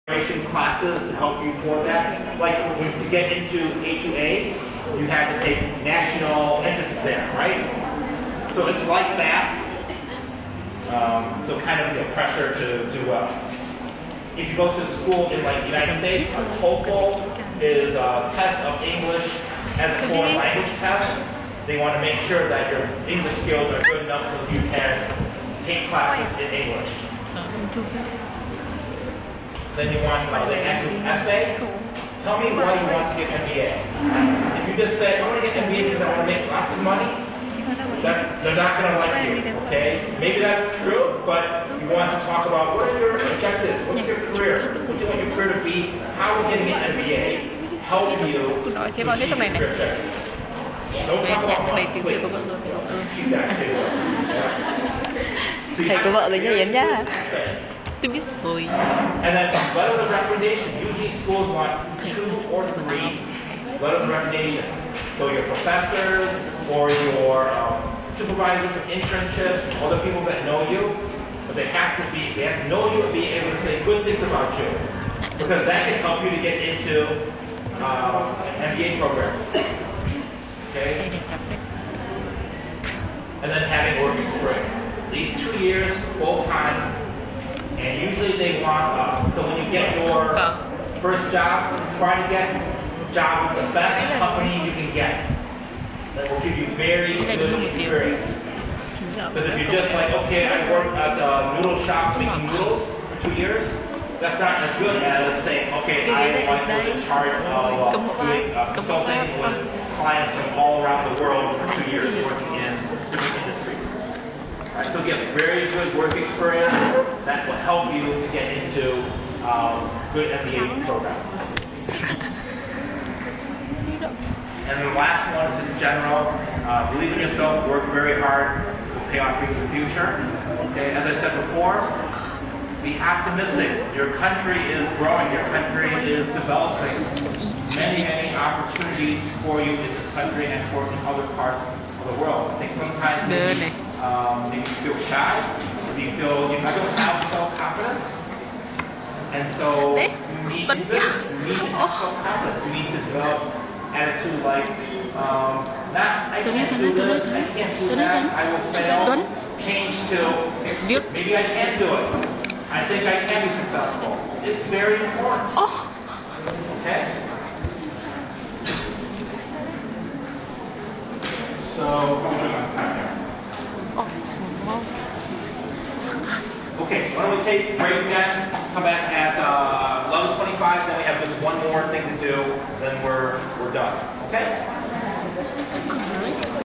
Lecture of Mr.Kuzuhara
This is a part in lecture of Mr.Kuzuhara about Organization Behavior in Hanoi university of Agriculture.